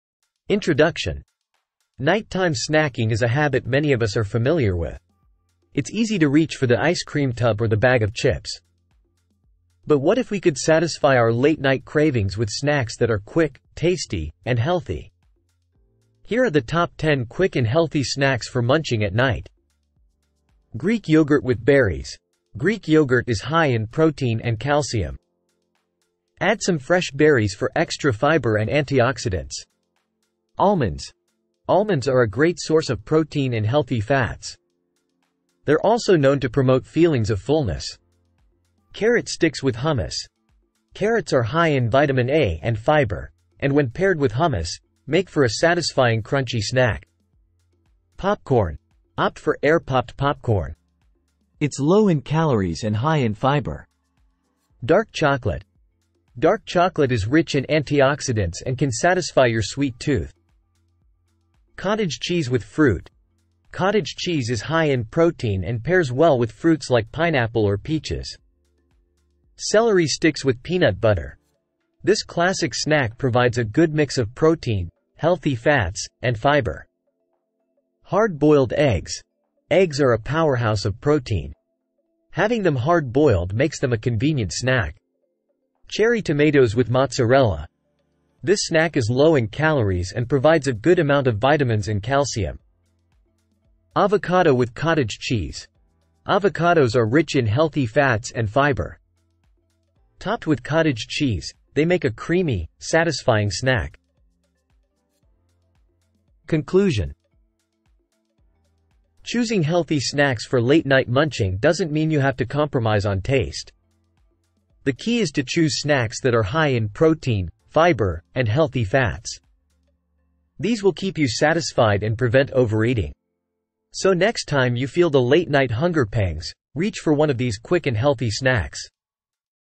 Top 10 Quick and Healthy Snacks for Munching at Night
Discover guilt-free munching options with these quick and healthy snacks. Satisfy your cravings without compromising your wellness goals.